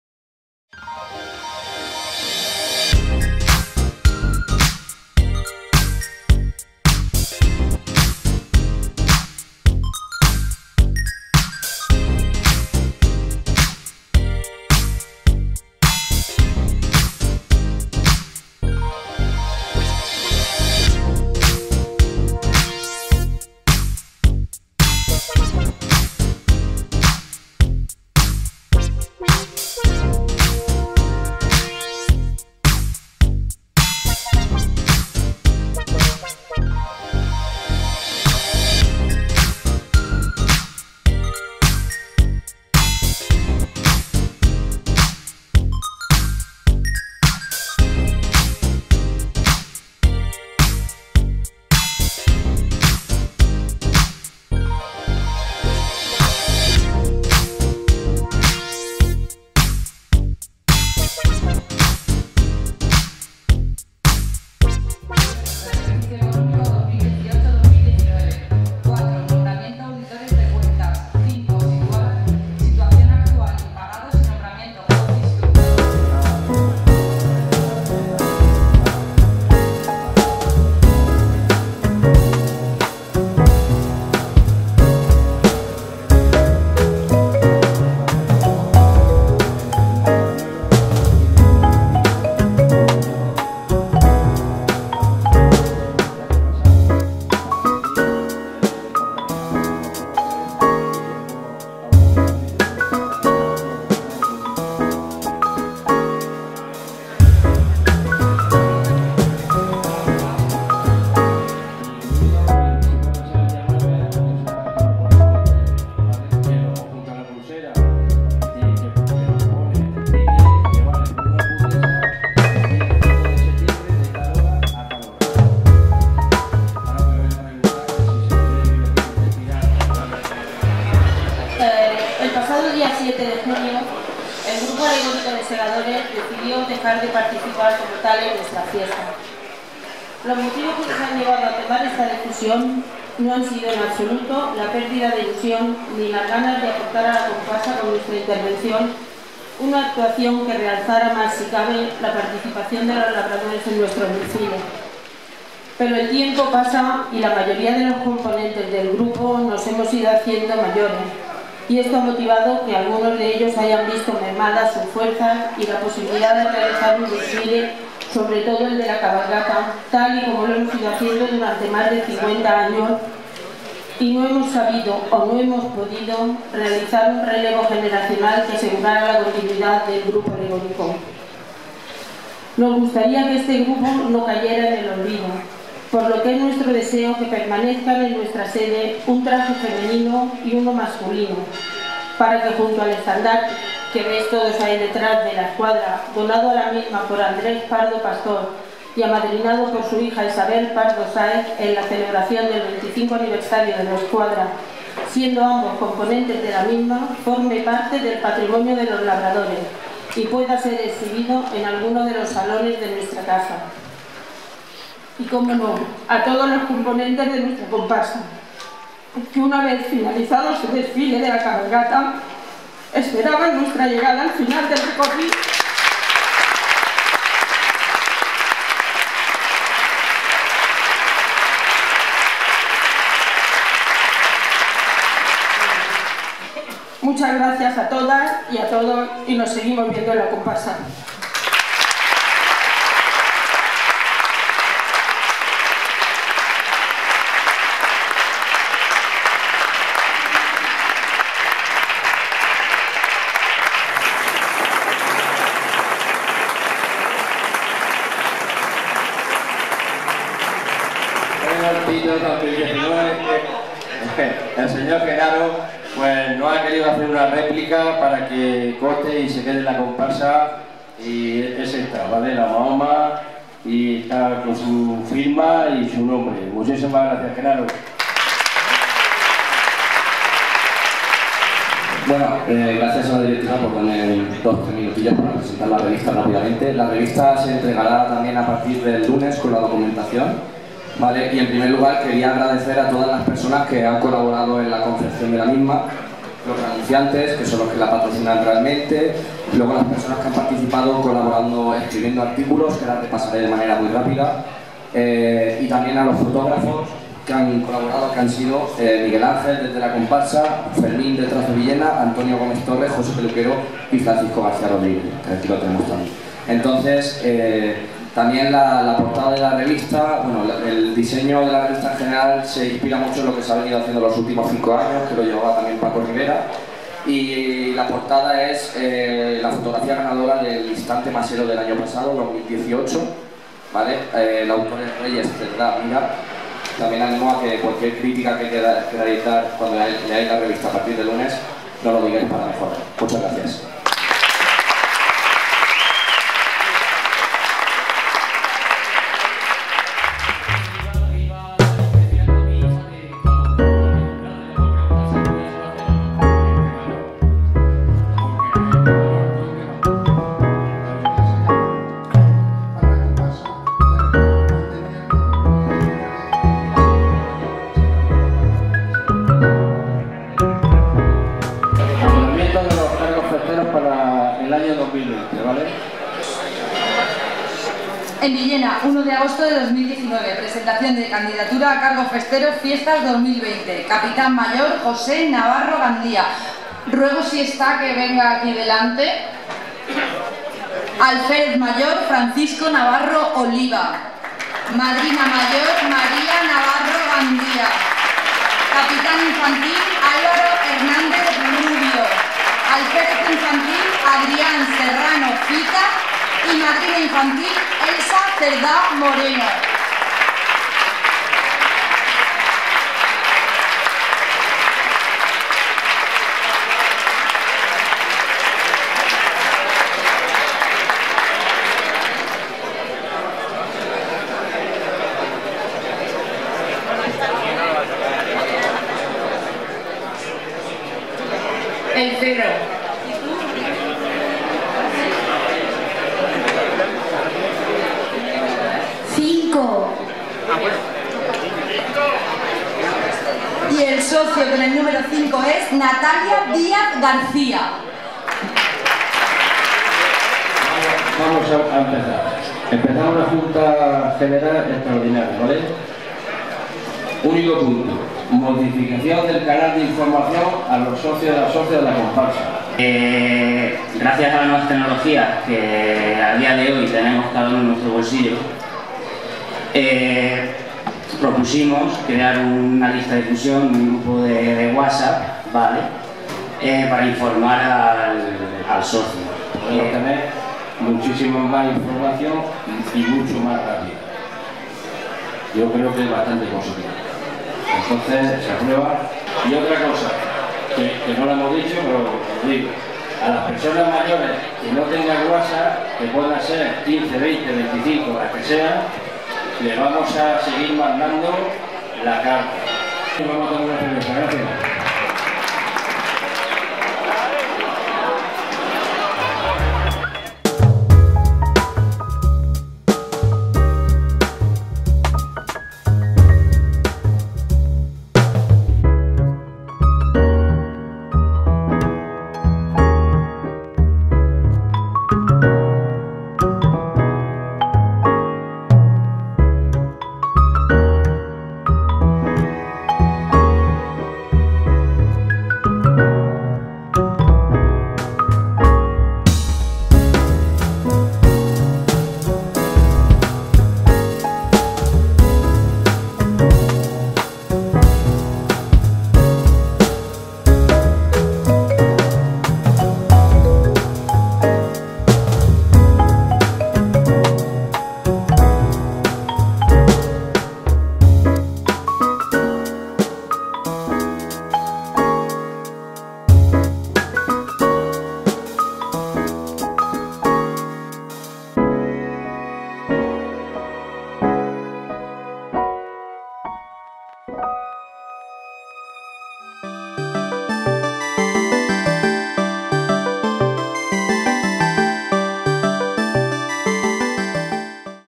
Labradores 2019. Junta del día 15
Esta junta con certeza es la más esperada por los maseros,  les anuncia que los días grandes de nuestras fiestas están aquí. Y como siempre familiaridad, buen ambiente que junto a las tradicionales habas y caracoles les hacen pasar una entrañable tarde.